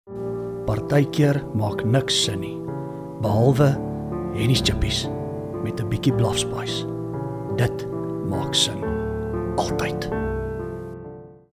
Hennie's bietjieBlaf Spice
Hennie’s BietjieBlaf Spice maak ALTYD sin 😉 Gooi dit op tjippies🍟, 'n kippie🍗 of 'n sappige stukkie steak🥩 Kry jou Bietjie Blaf Spice by jou local Hennie's, of by jou naaste Slaghuis/Deli en Supermark*
Sien jou by Hennie's
*As jy of 'n pel 'n slaghuis, deli of supermark besit en wil graag van ons produkte aan hou, kontak gerus ons verspreiders by 📧
👉Nuwe website update launch binnekort met jou naaste stockist lys